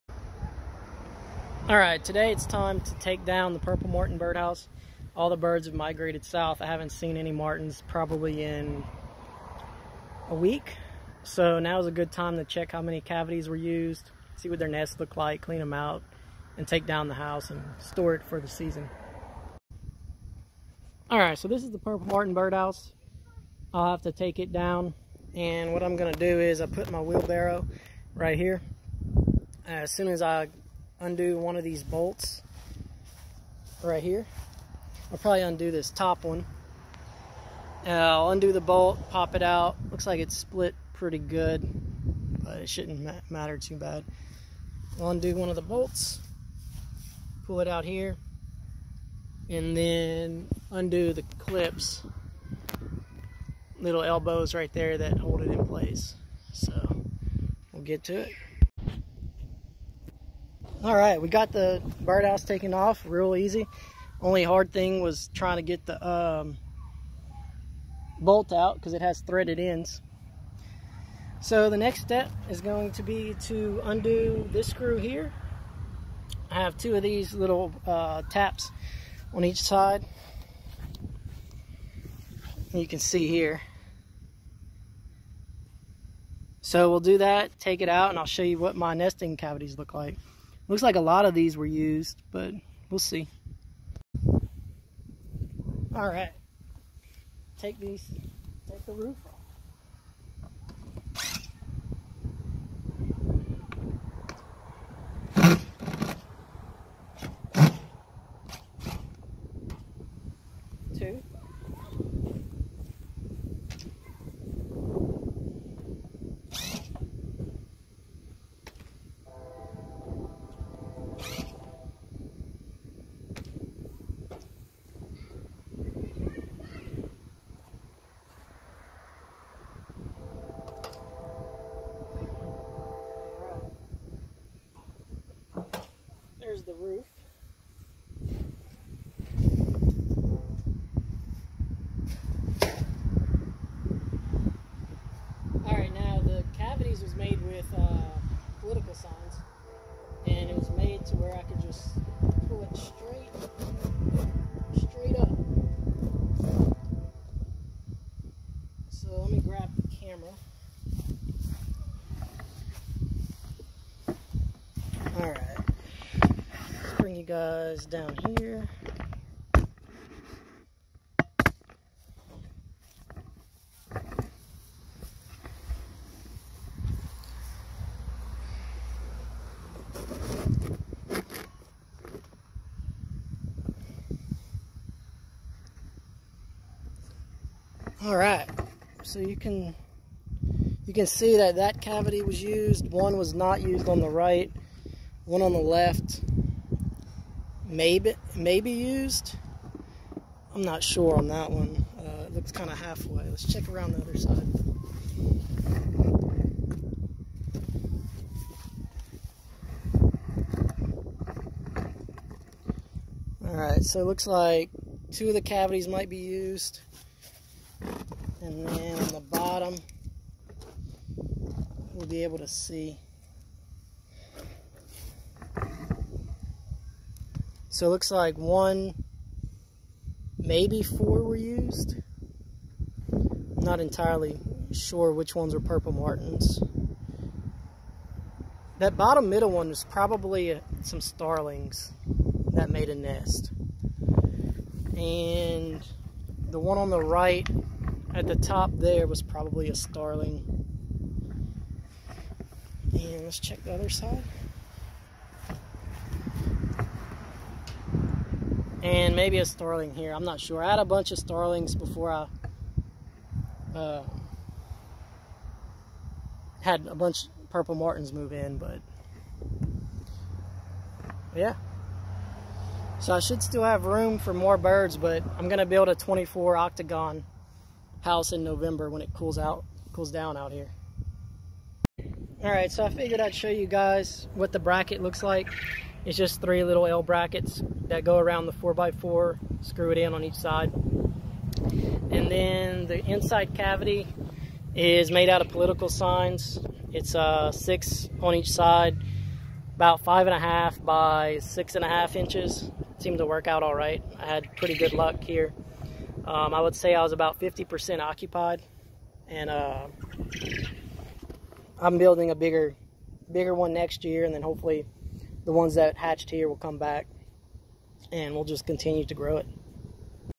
FIRST Purple Martin House Cleanout
Every year you have to remove and clean out the Purple Martin houses. This is how my very FIRST year went.